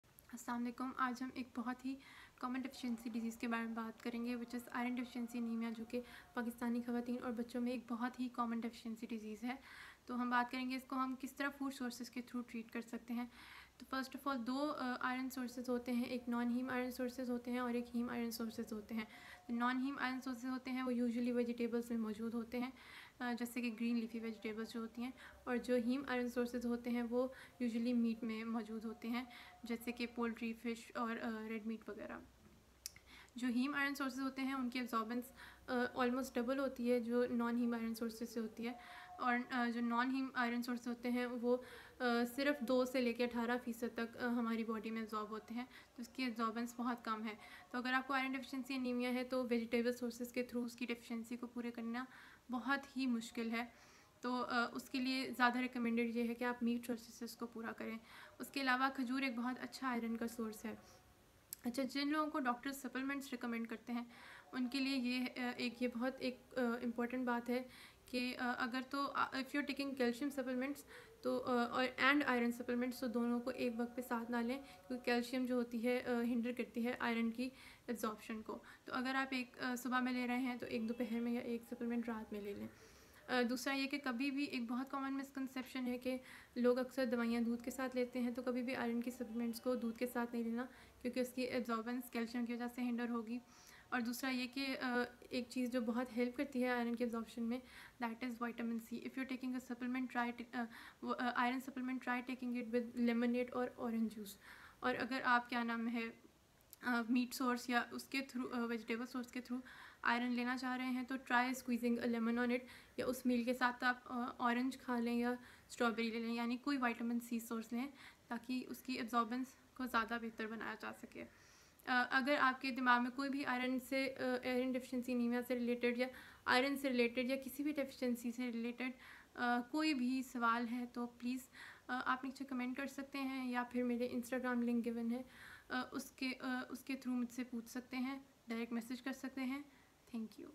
خون کی کمی کا آسان علاج
Wonderful video courtesy of Consultant Nutritionist Tehreem Fatima on iron deficiency anaemia and its treatment through nutrition. It must be noted that over half of the female and children population of Pakistan suffer from this issue and by employing the advice of Madam Tehreem we can prevent them many complications related to it.